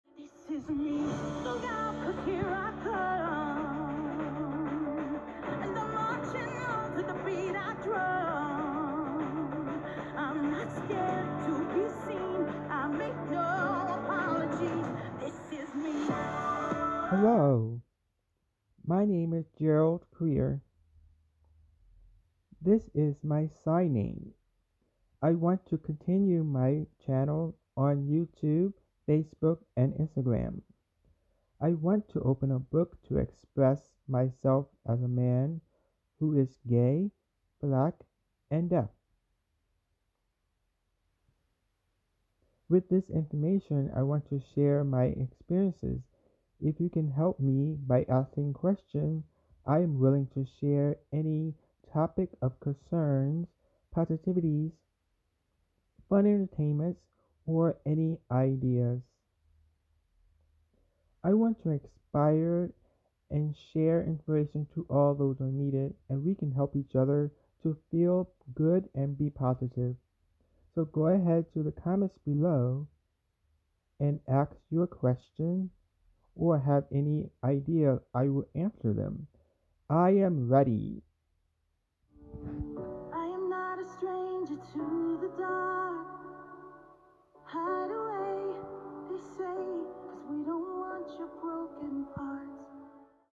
Black Gay Deaf Intro
This video is an introduction about me being Black Gay Deaf. I am trying to get back on Vlogging so that I can continue to inspire the world with my talent and what I am inspired to do. I have a strong passion to educate my ASL community and Deaf communities on Deaf Eduction and the needs of Deaf children and families. I believe I am ready to do whatever it takes to put myself In the public to show what I can do to help and engage. I am all about collaboration, teaching and having fun! There will be more to come from Signing Songs, storytelling, Poetry, and much more!